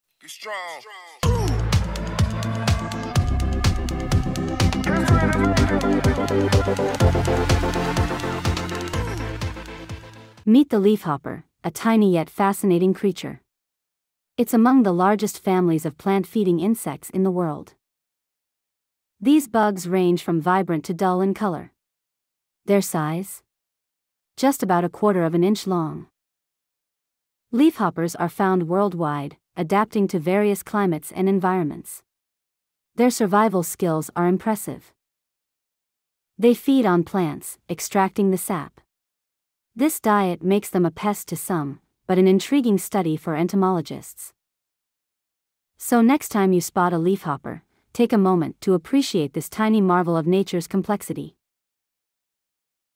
"Tiny Marvels: Exploring the Extraordinary World of Leafhoppers"
Step into the miniature world of leafhoppers, where vibrant colors and incredible acrobatics await. In this one-minute documentary, we'll take you on a journey through their intricate lives, exploring their ecological importance and the hidden beauty of their diverse species. Get ready to be amazed by the tiny marvels that play a vital role in our natural world!